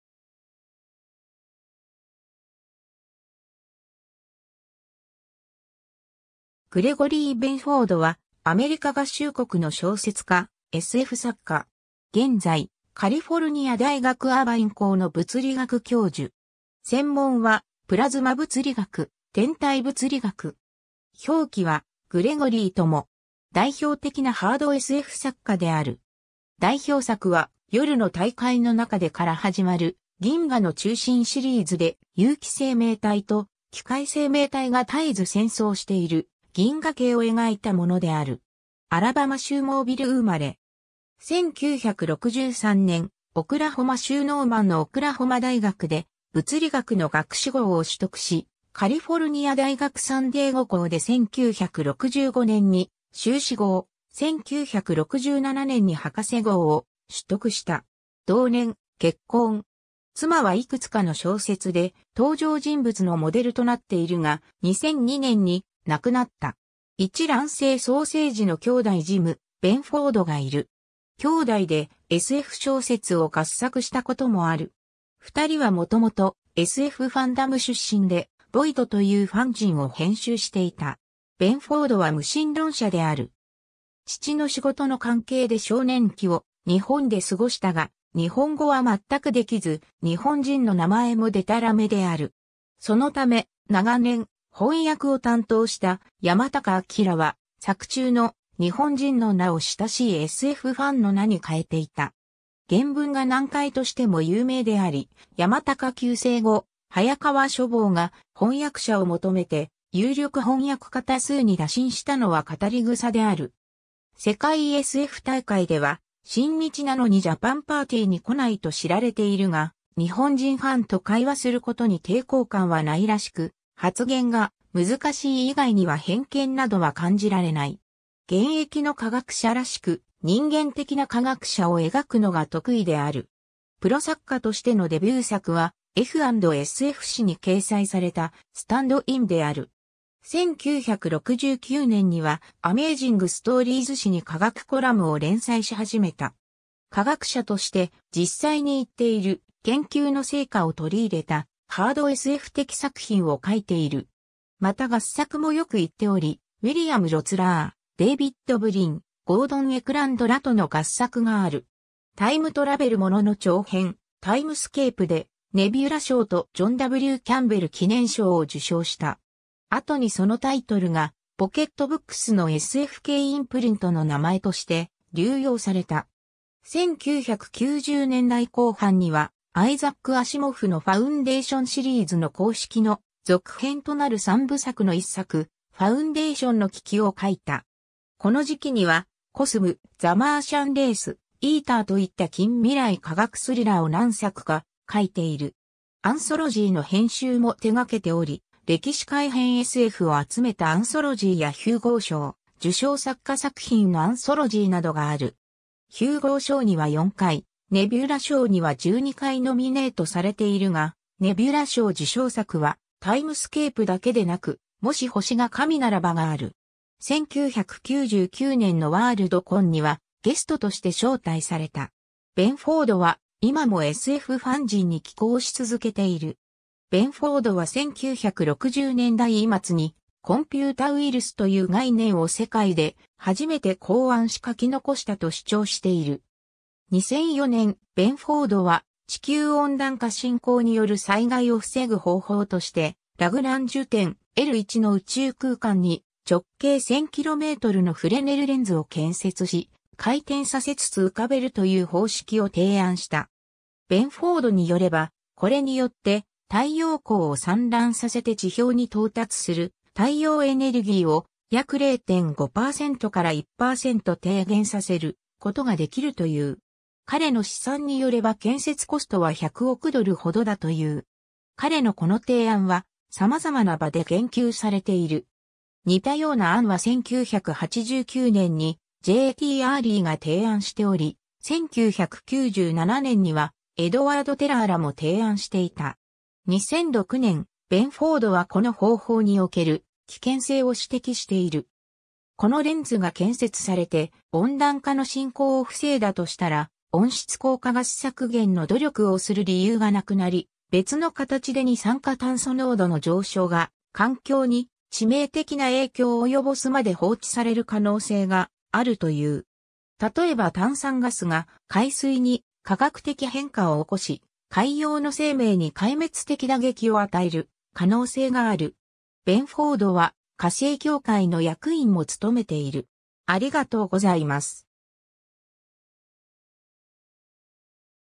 グレゴリー・ベンフォード
グレゴリー・ベンフォード（Gregory Albert Benford, 1941年1月30日 - ）は、アメリカ合衆国の小説家、SF作家。
 現在、カリフォルニア大学アーバイン校の物理学教授。
専門はプラズマ物理学、天体物理学。
表記はグレゴリイとも。
 代表的なハードSF作家である。
代表作は『夜の大海の中で』(1977) から始まる「銀河の中心」シリーズで、有機生命体と機械生命体が絶えず戦争している銀河系を描いたものである。
 アラバマ州モービル生まれ。
1963年、オクラホマ州ノーマンのオクラホマ大学で物理学の学士号を取得し、カリフォルニア大学サンディエゴ校で1965年に修士号、1967年に博士号を取得した。
同年、結婚。
妻はいくつかの小説で登場人物のモデルとなっている（例えば『時の迷宮』のヒロイン）が、2002年に亡くなった。
 一卵性双生児の兄弟ジム・ベンフォードがいる。
兄弟でSF小説を合作したこともある。
2人はもともとSFファンダム出身で、Void というファンジンを編集していた。
ベンフォードは無神論者である。
 父の仕事の関係で少年期を日本で過ごしたが、日本語は全くできず、日本人の名前もデタラメである。
そのため、長年、翻訳を担当した山高昭は作中の「日本人」の名を親しいSFファンの名に変えていた。
原文が難解としても有名であり、山高急逝後、早川書房が翻訳者を求めて有力翻訳家多数に打診したのは語り草である。
 世界ＳＦ大会では、親日なのにジャパンパーティーに来ないと知られているが、日本人ファンと会話する事に抵抗感は無いらしく、発言が難しい以外には偏見などは感じられない。
 現役の科学者らしく、「人間的な科学者」を描くのが得意である。
 プロ作家としてのデビュー作はF&SF誌（1965年6月）に掲載された "Stand-In" である。
1969年には、アメージング・ストーリーズ誌に科学コラムを連載し始めた。
 科学者として実際に行っている研究の成果を取り入れたハードSF的作品を書いている。
また合作もよく行っており、ウィリアム・ロツラー(en)、デイヴィッド・ブリン、ゴードン・エクランド(en) らとの合作がある。
タイムトラベルものの長編『タイムスケープ』(1980)で、ネビュラ賞とジョン・W・キャンベル記念賞を受賞した。
後にそのタイトルが Pocket Books のSF系インプリントの名前として流用された。
1990年代後半には、アイザック・アシモフのファウンデーションシリーズの公式の続編となる三部作の1作『ファウンデーションの危機』を書いた。
この時期には Cosm (1998)、The Martian Race (1999)、Eater (2000) といった近未来科学スリラーを何作か書いている（日本語未訳）。
 アンソロジーの編集も手がけており、歴史改変SFを集めたアンソロジーやヒューゴー賞受賞作家作品のアンソロジーなどがある。
 ヒューゴー賞には4回、ネビュラ賞には12回ノミネートされているが、ネビュラ賞受賞作は『タイムスケープ』だけでなく、「もし星が神ならば」（長編『もし星が神ならば』の第2部の元になった中編）がある（エクランドとの合作）。
 1999年のワールドコン（メルボルンで開催）にはゲストとして招待された。
ベンフォードは今もSFファンジンに寄稿し続けている。
 ベンフォードは1960年代末にコンピュータウイルスという概念を世界で初めて考案し書き残したと主張している。
 2004年、ベンフォードは地球温暖化進行による災害を防ぐ方法として、ラグランジュ点L1の宇宙空間に直径1000kmのフレネルレンズを建設し、回転させつつ浮かべるという方式を提案した。
ベンフォードによれば、これによって太陽光を散乱させて地表に到達する太陽エネルギーを約0.5%から1%低減させることができるという。
彼の試算によれば建設コストは100億ドルほどだという。
彼のこの提案は様々な場で言及されている。
似たような案は1989年に J. T. Early が提案しており、1997年にはエドワード・テラーらも提案していた。
2006年、ベンフォードはこの方法における危険性...